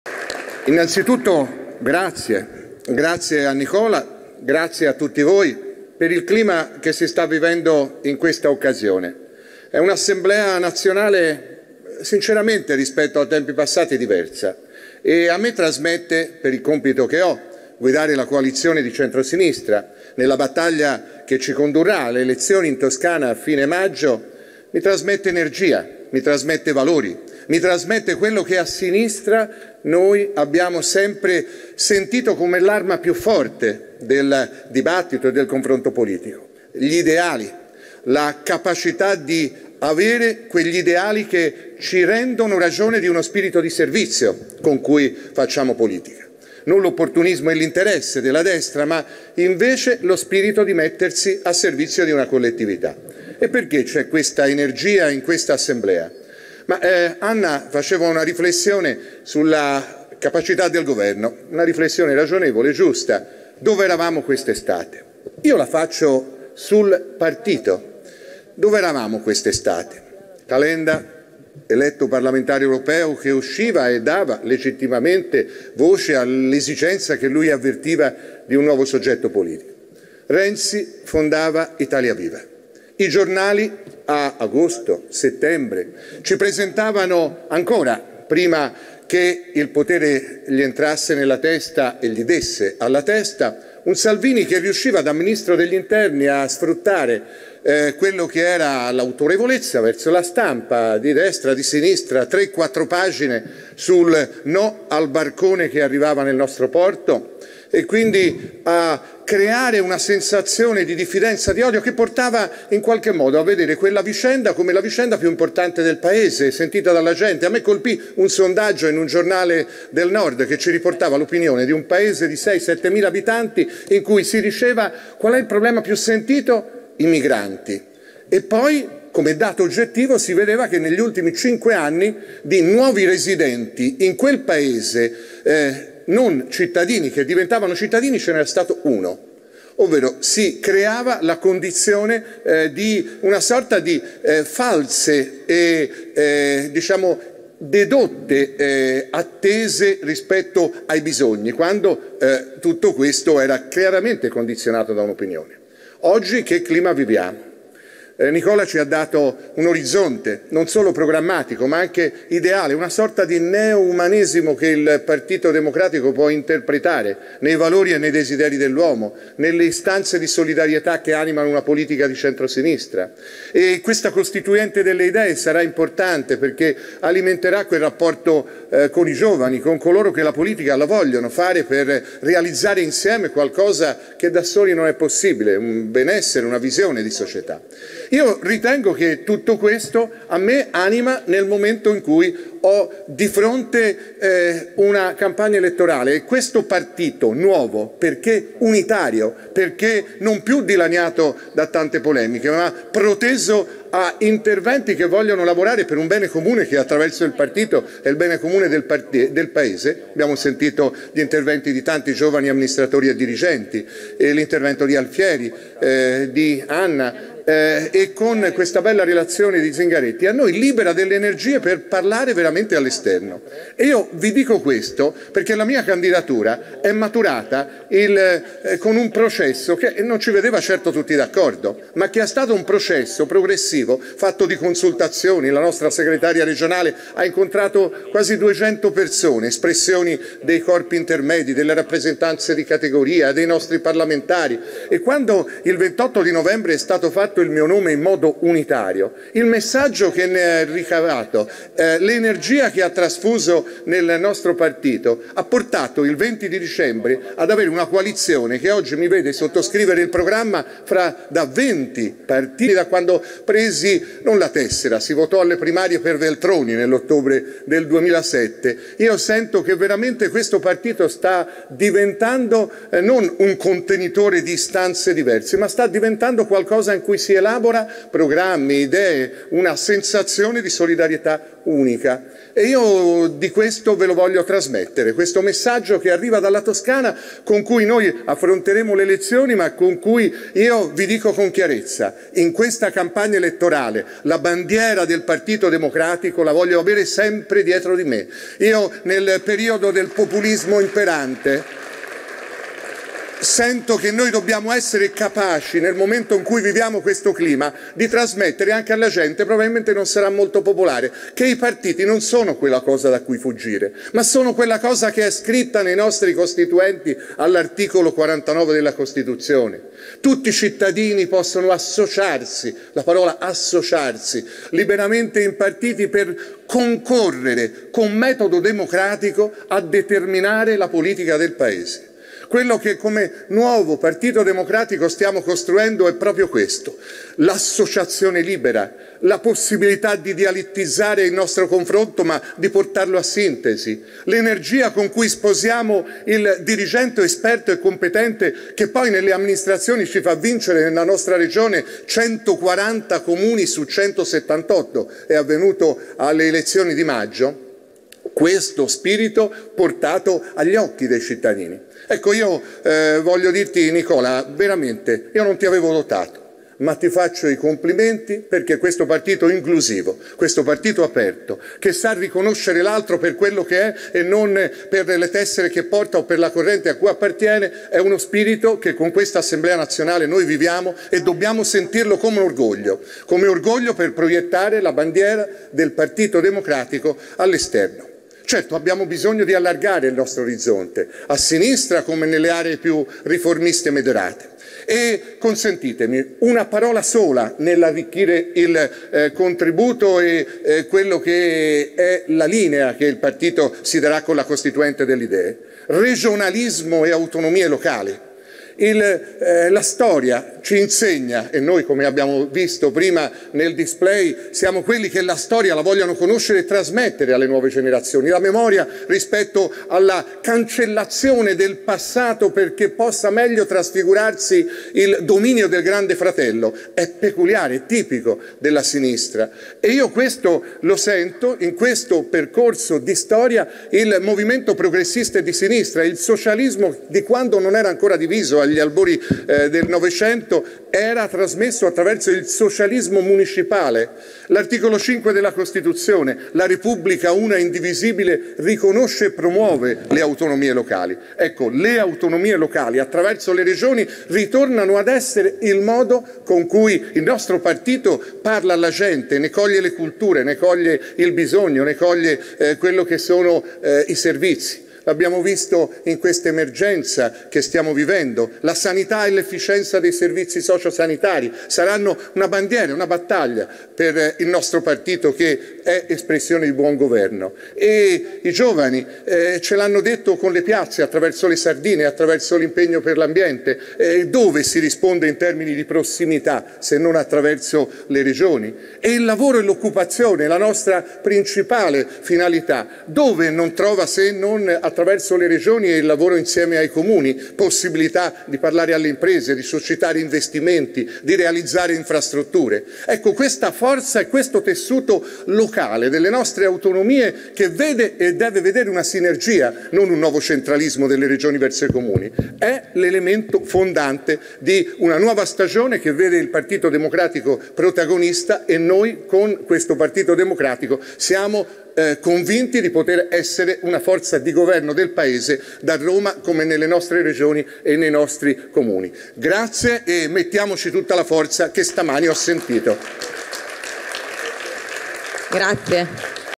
Assemblea Nazionale PD - Eugenio Giani
Eugenio Giani interviene all'Assemblea Nazionale PD - Roma, 22 febbraio 2020